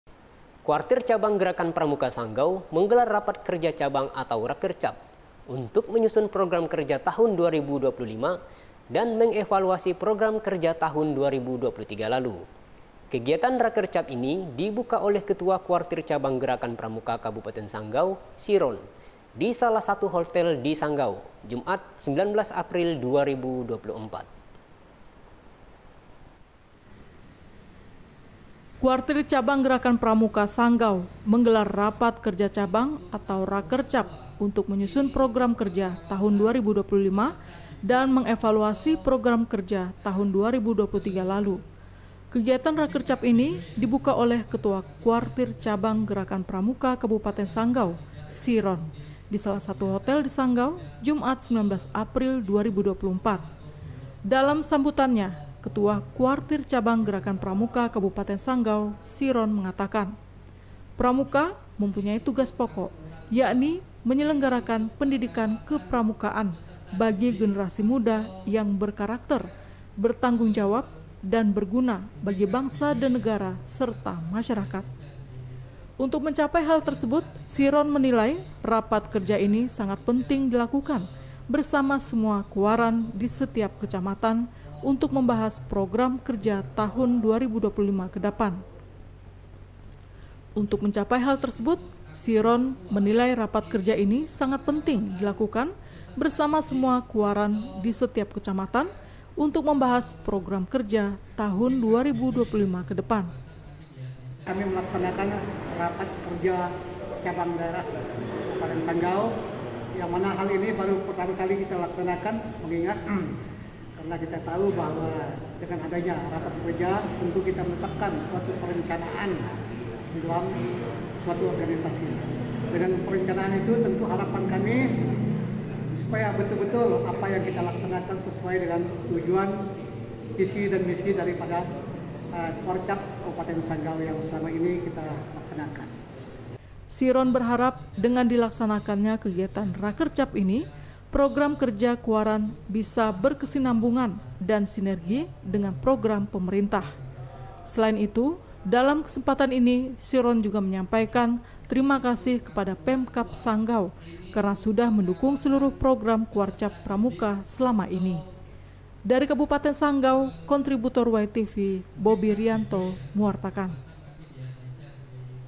KWARTIR PRAMUKA SANGGAU GELAR RAKERCAB MENYUSUN PROGRAM KERJA TAHUN 2025
Berita Warta Ruai, Tgl 22 APRIL 2024

Saksikan Warta Ruai Malam, Melalui live streaming kami di chanel youtube : Live Streaming Ruai Televisi, setiap hari mulai pukul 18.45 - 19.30 WIB.